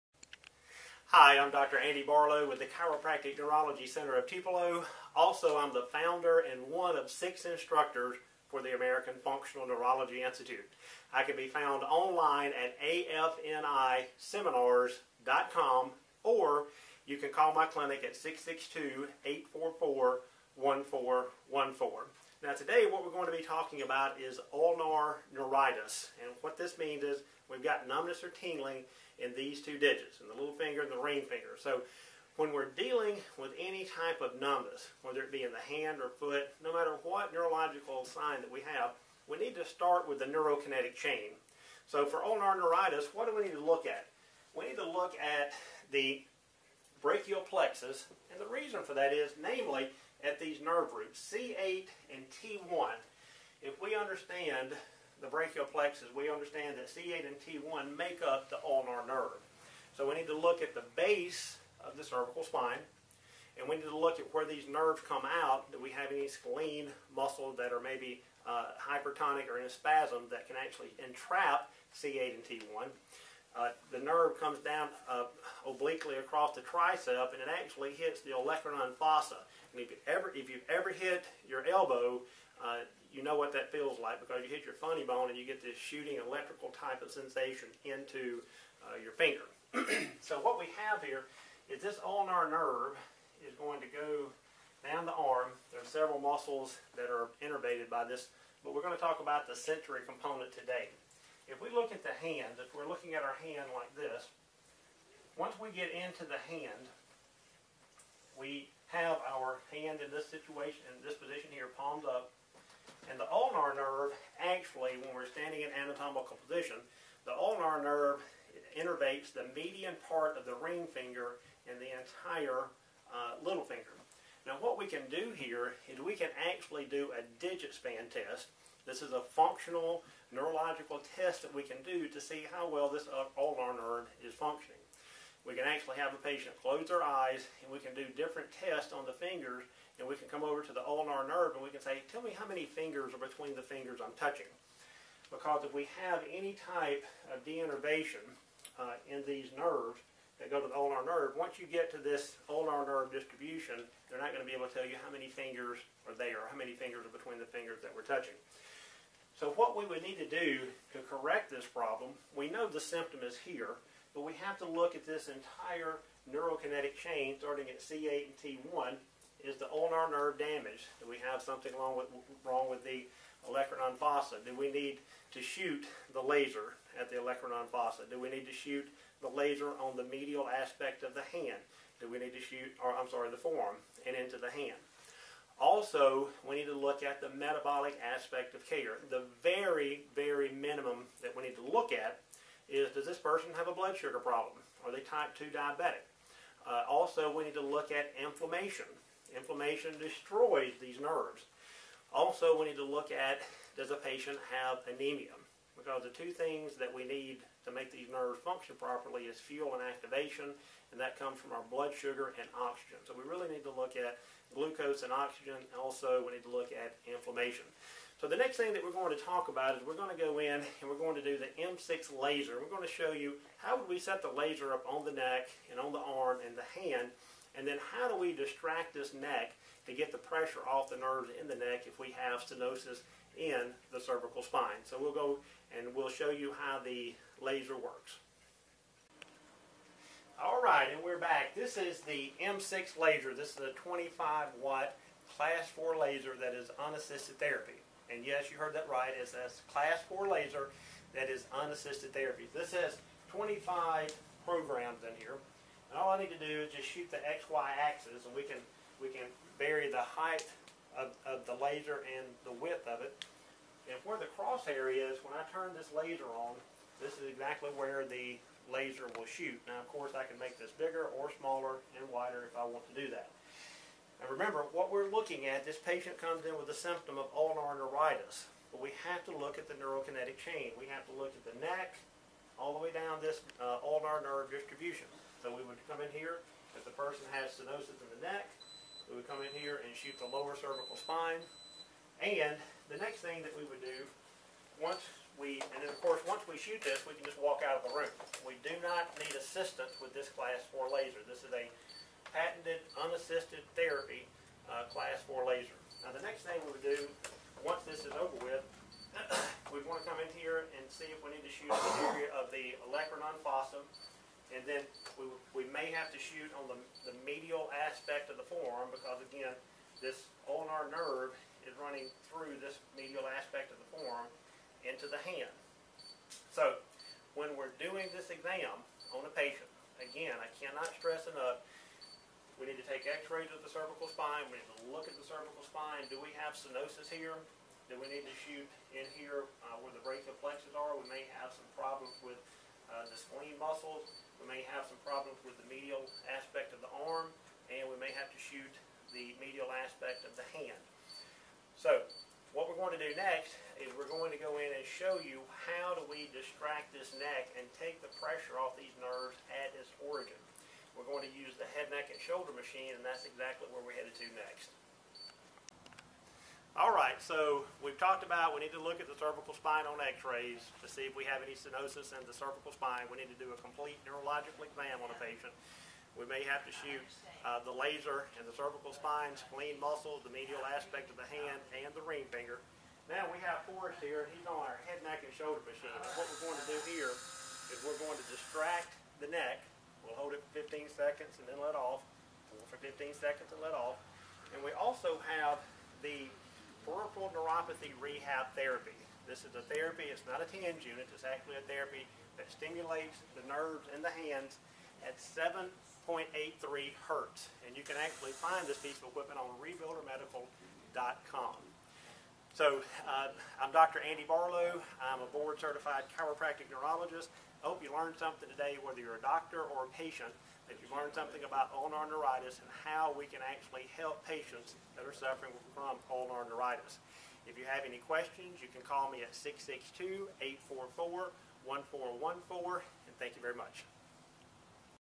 Dr. Andy Barlow discusses ulnar neuritis
- Dr. Andy Barlow discusses the neuro-pathways associated with Ulnar Neuritis and treatment options.